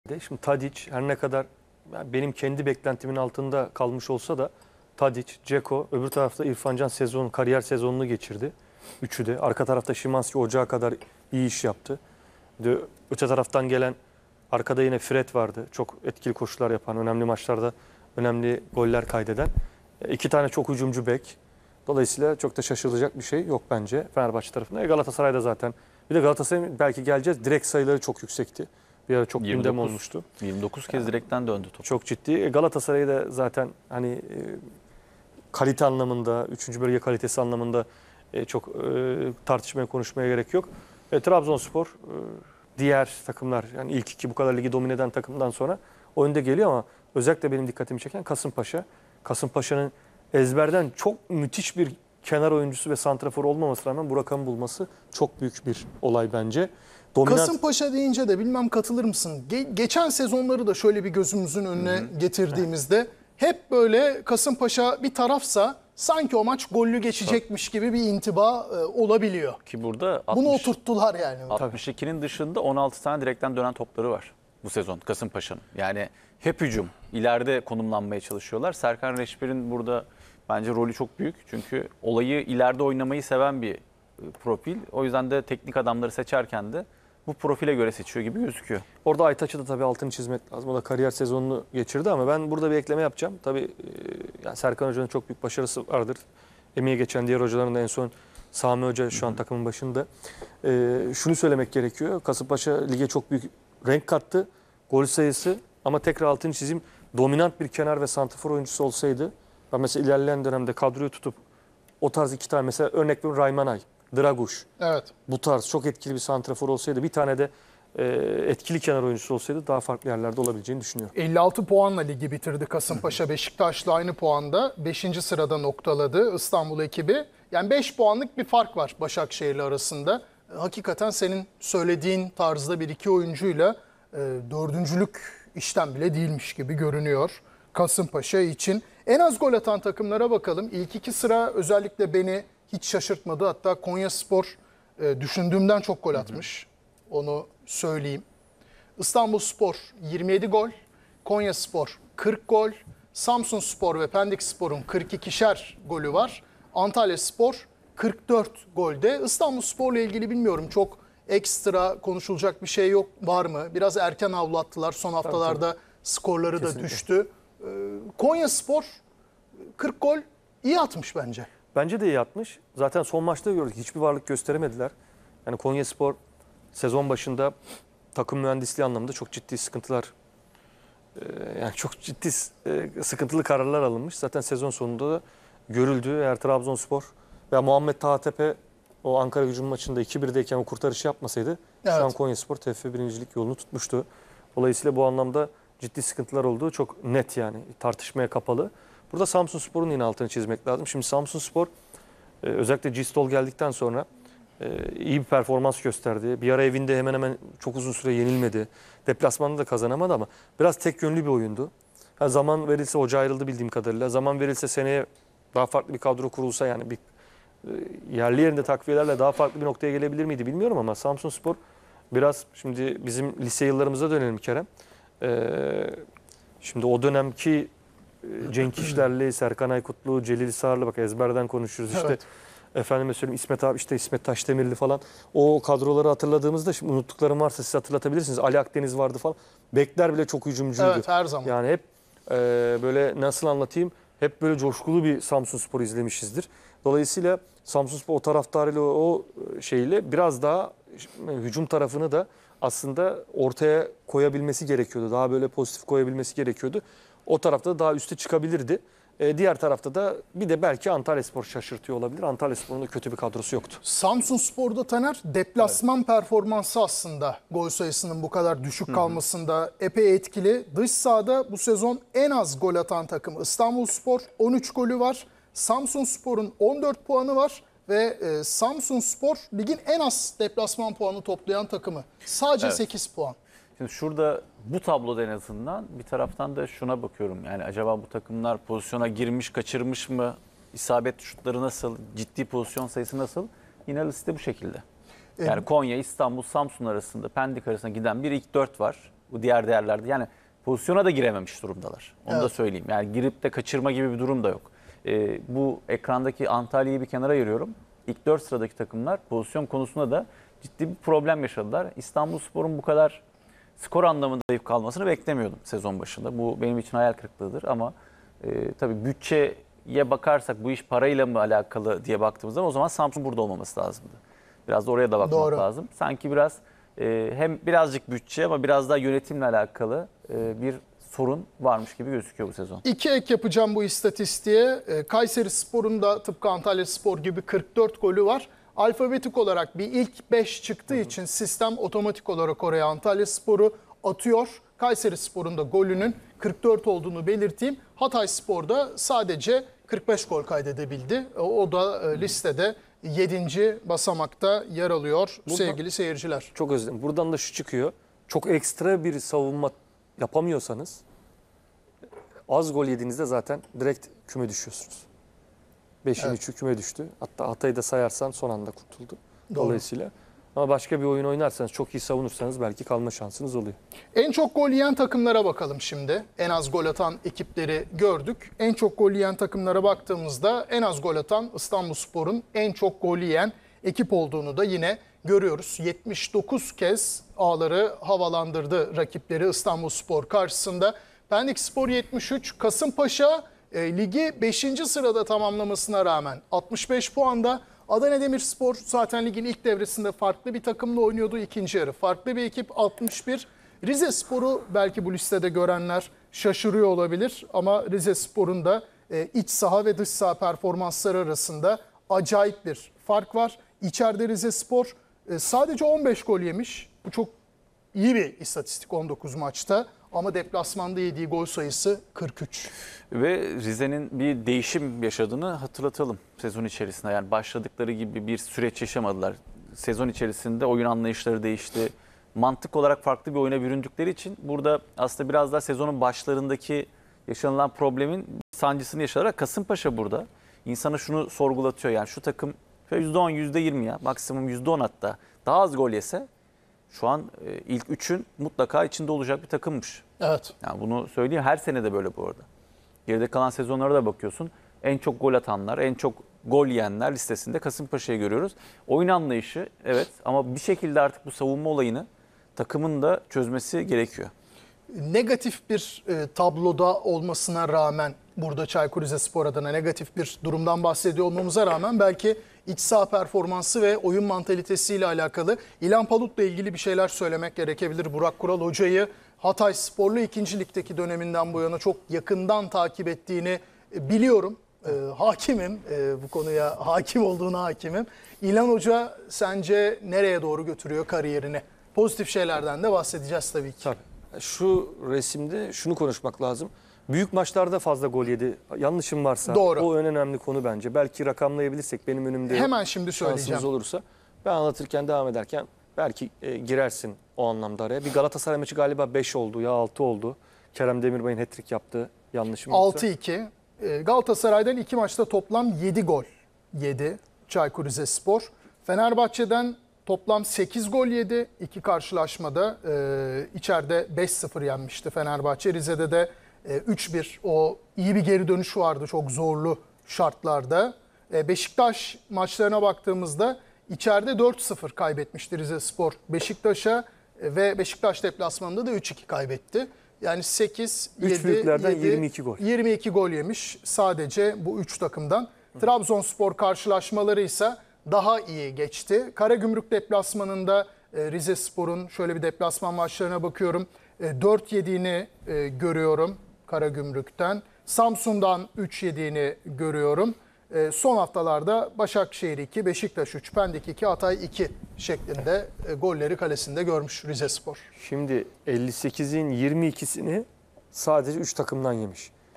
Burak Kural: "Fenerbahçeli İrfan Can Kahveci Kariyer Sezonunu Geçirdi" / A Spor / Süper Lig Analiz
Burak Kural: "Fenerbahçeli İrfan Can Kahveci Kariyer Sezonunu Geçirdi" / A Spor / Süper Lig Analiz / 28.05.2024